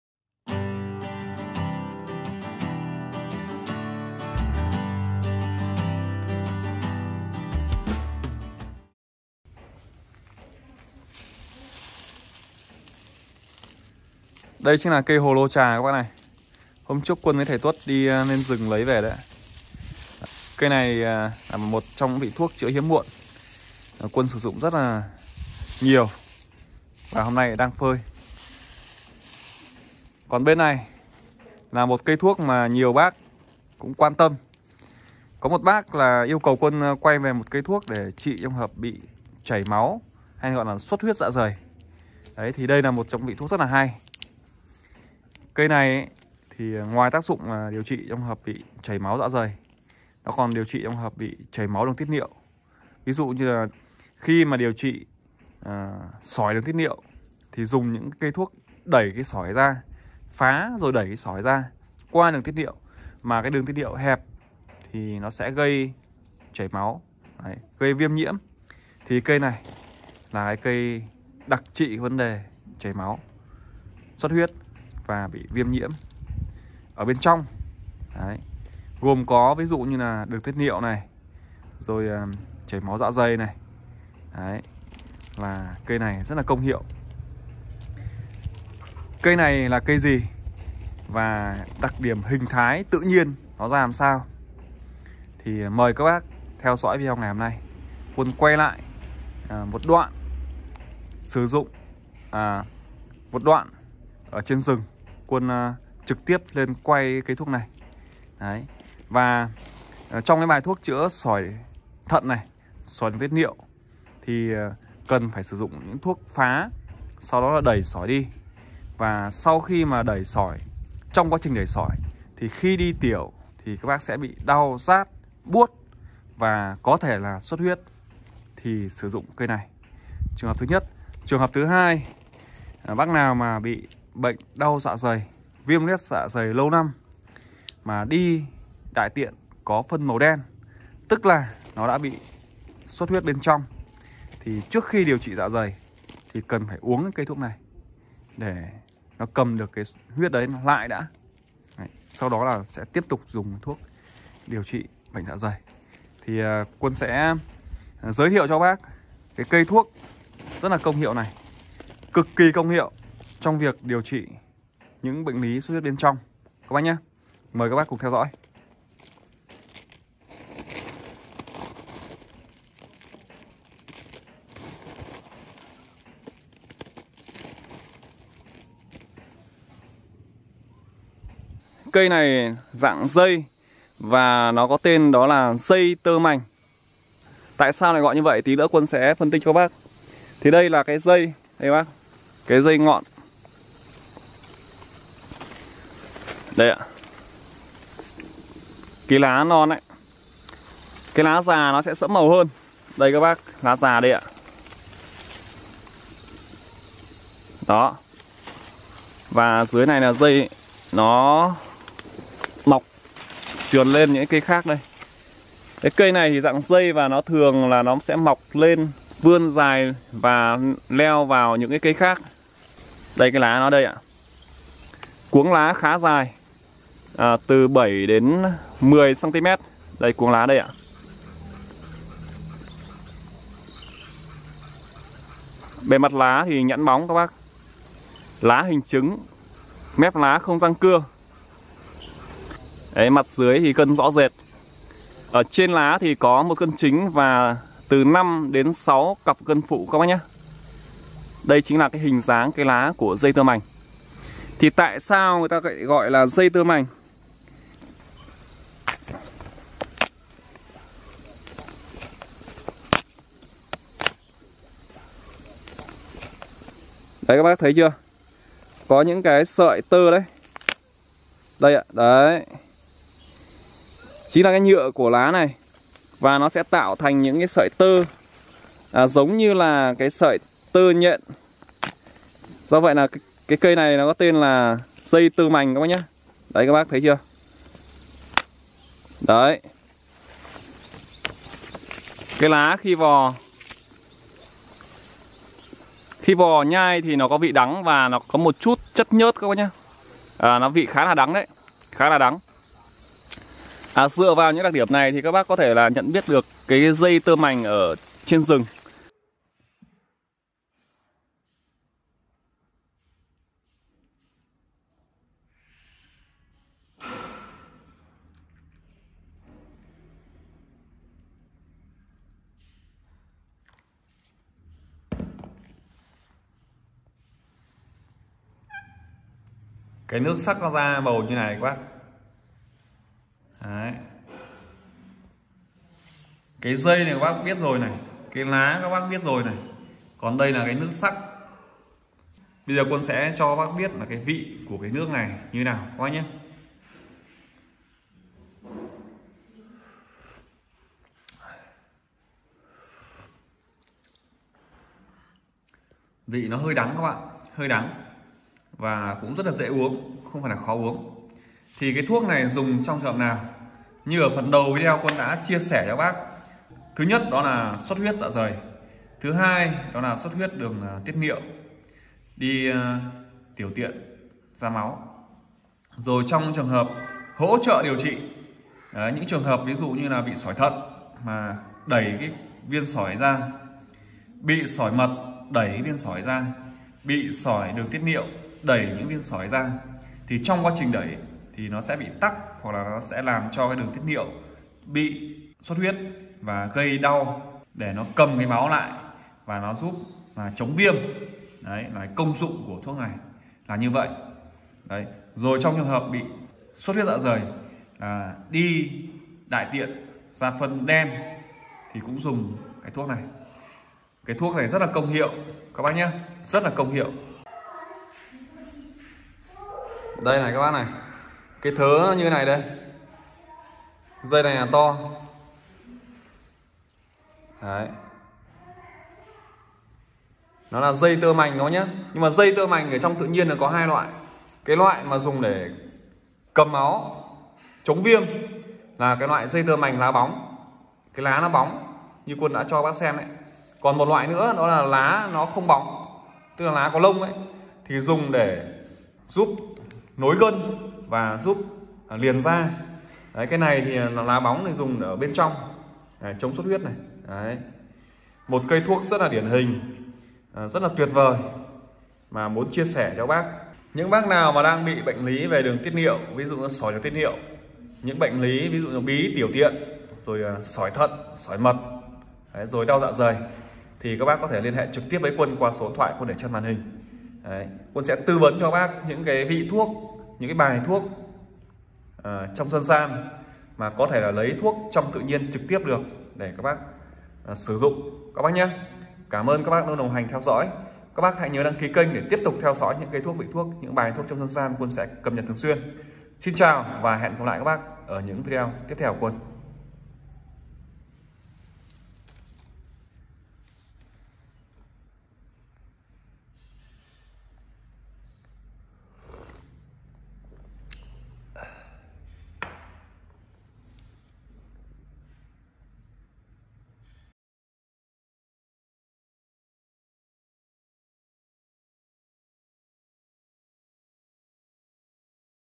Dây tơ mành trị Sỏi đường tiết niệu, xuất huyết dạ dày rất quý | Thaoduoc.net
Dây tơ mành dùng để trị xuất huyết dạ dày, viêm đường tiết niệu, hoặc khi đẩy phá sỏi (thận, mật, tiết niệu) viên sỏi chui ra gây đau, chảy máu thì dùng cây này sắc nước uống.
👉 Nhân trần cây thuốc tốt cho gan và những lưu ý khi sử dụng hàng ngày
👉 Đào Thải Độc Tố Tích Tụ Trong Cơ Thể Chỉ Bằng Nắm Lá Tắm Của Người Vùng Cao